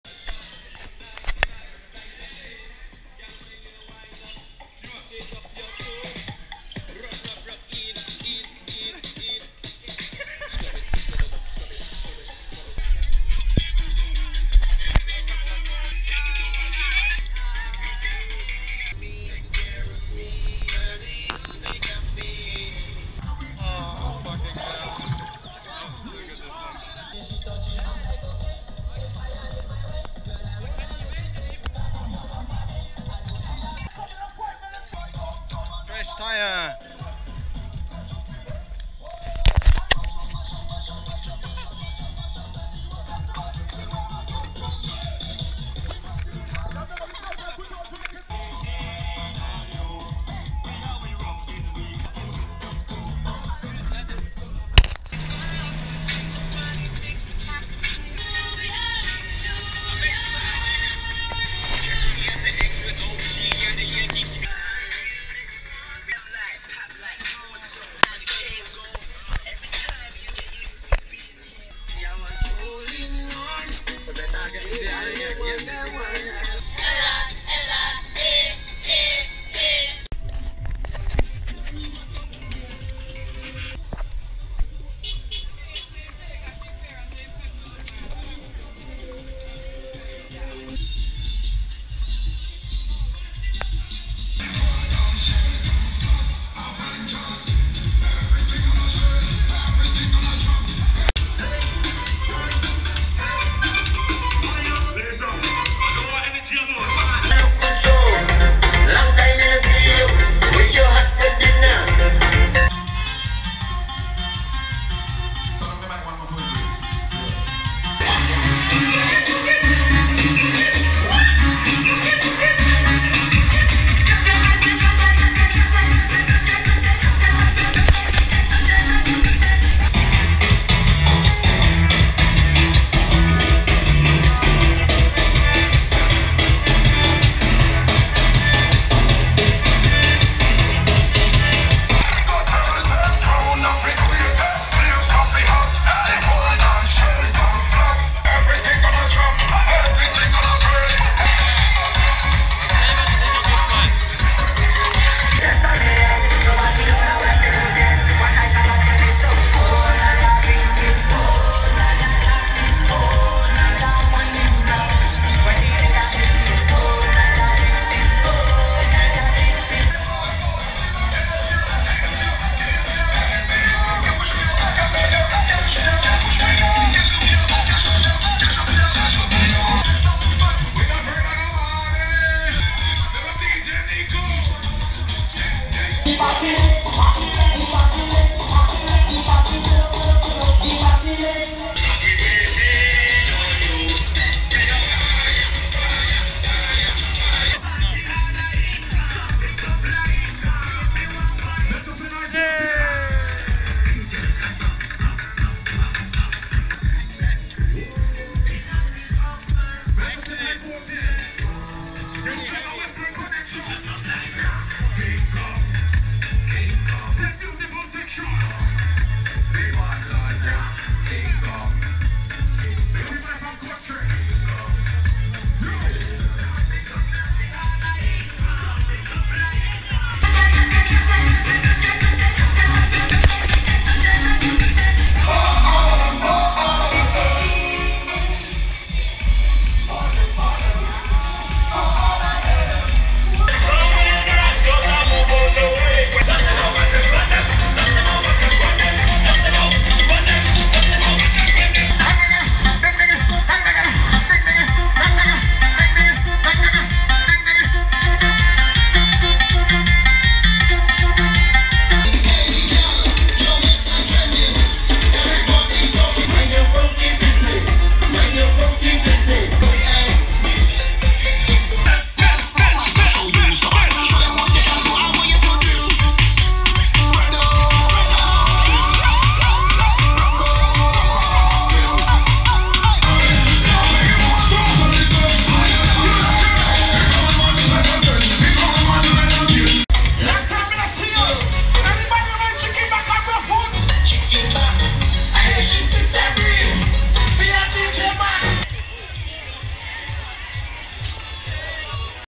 Carnival 2018 | Vincy Mas - St. Vincent and the Grenadines
Fantastic Vincy Mas 2018!!!
Amazing people and great atmosphere!!! Here are my best moments!